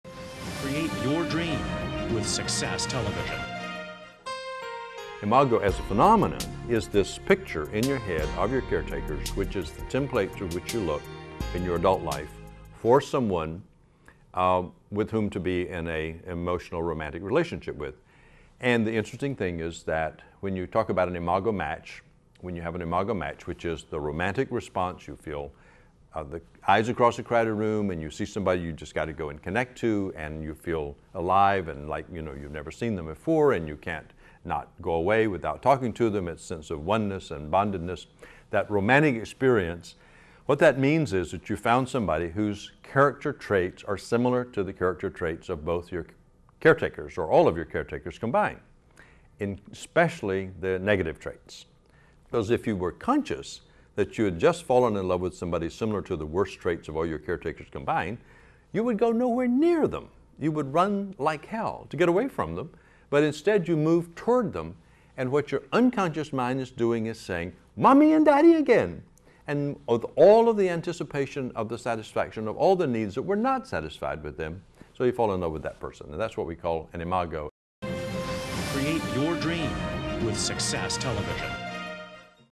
Who Is Your Perfect Lover?
Harville Hendrix, best selling author of "Keeping the Love Your Find" and founder of Imago Relationship Therapy, explains what "Imago" means and how it relates to your soul mate.